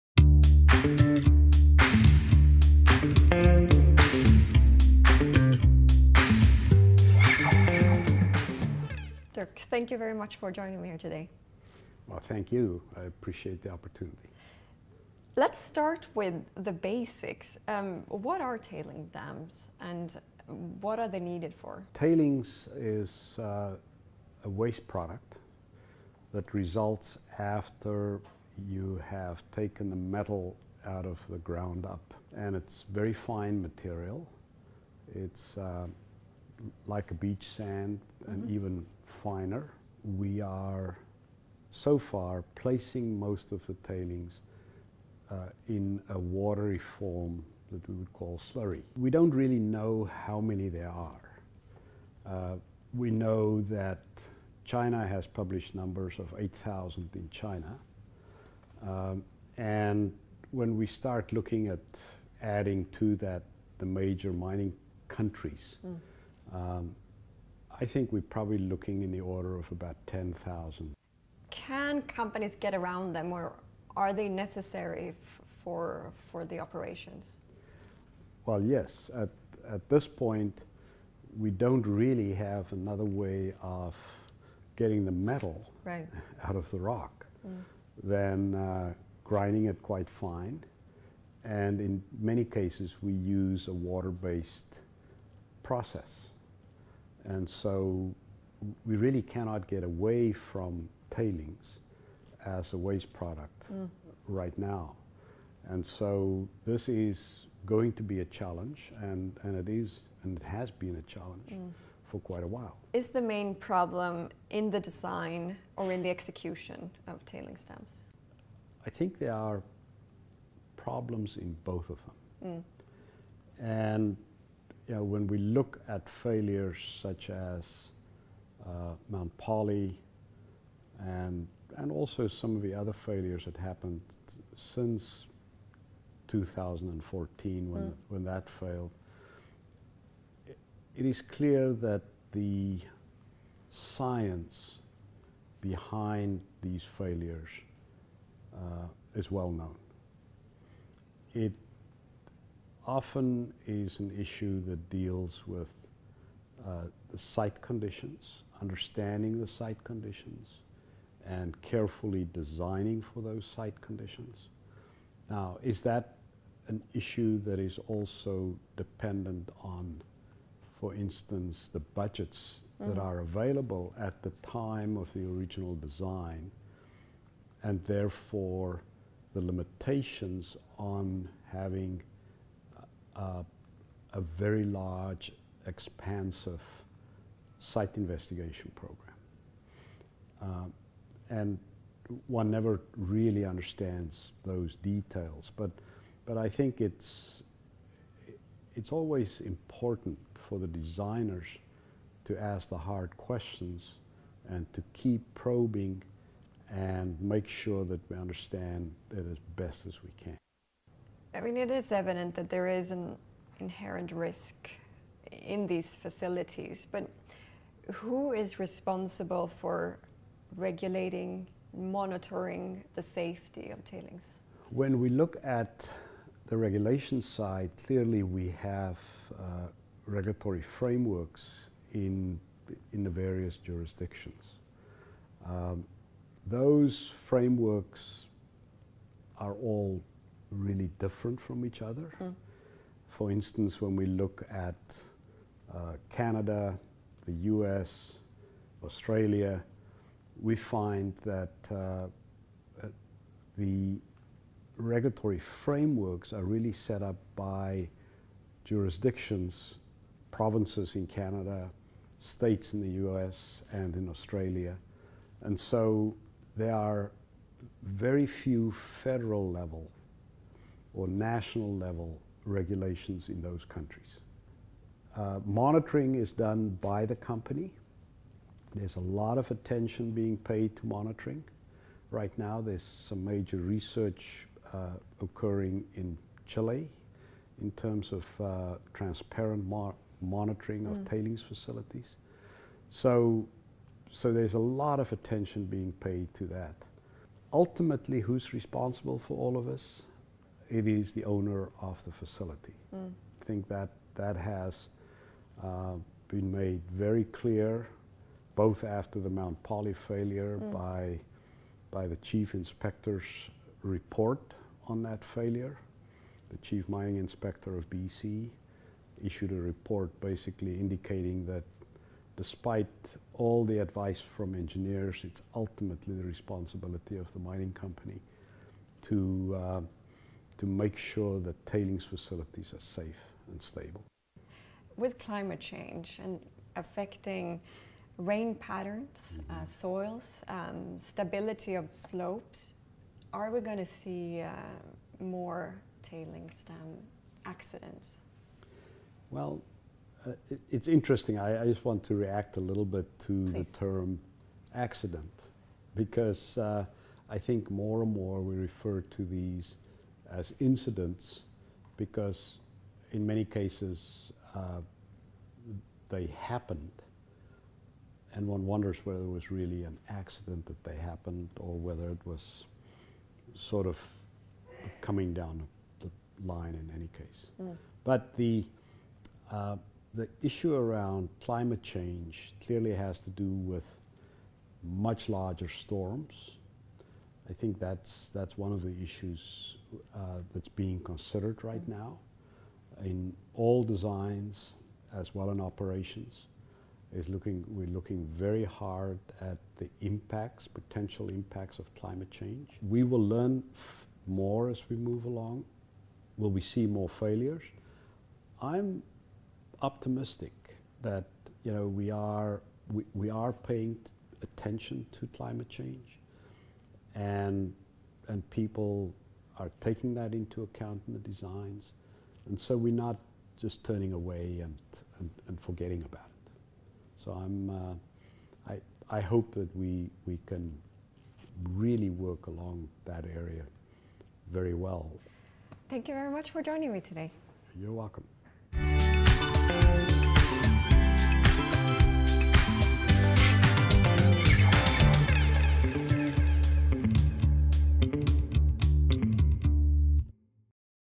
Tailings Dams: Why Do Failures Keep Happening? | Highgrade Talks with Dirk Van Zyl
The tailings dam collapse in Brumadinho, Brazil, in early 2019 put tailing management on top of the natural resources agenda. Why do these dam failures keep happening? We talk to Professor Dirk Van Zyl, an expert in tailings management.